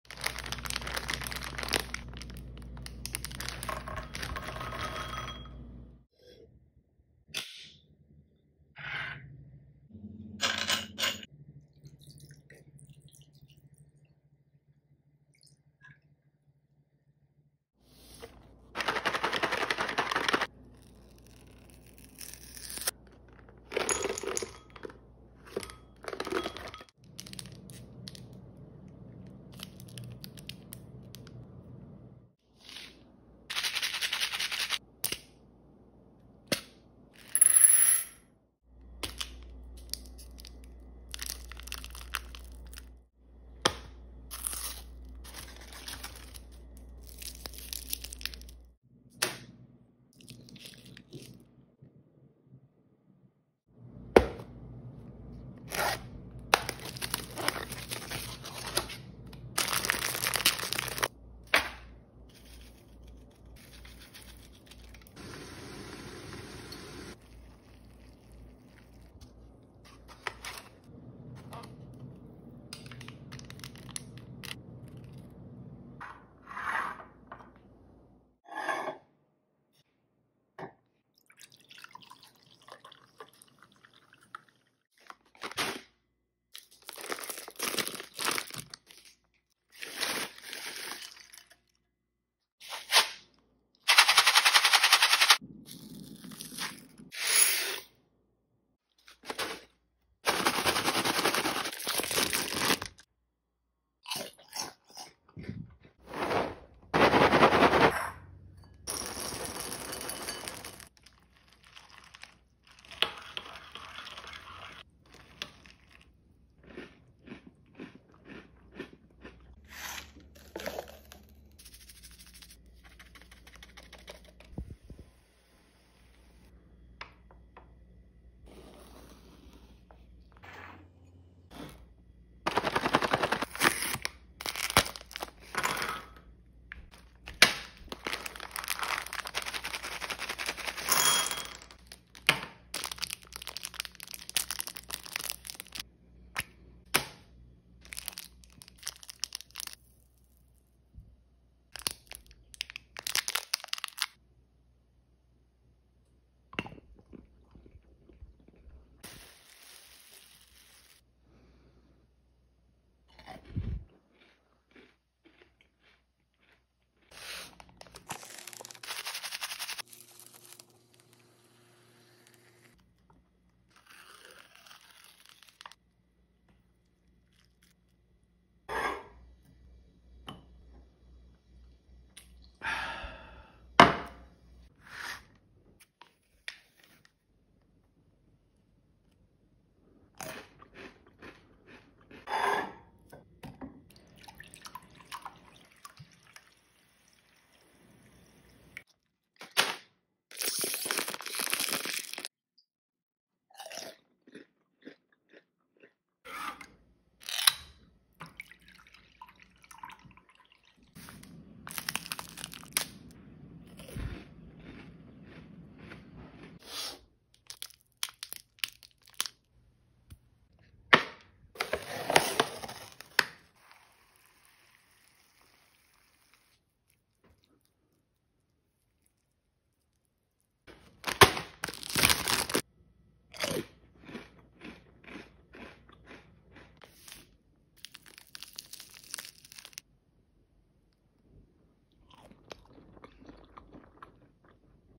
~ SATISFYING FOOD ASMR ~ // ~ COMPILATION ~ // PART 2
ASMR compilation full of triggers and tingles. I hope this relaxes you and makes you feel at peace!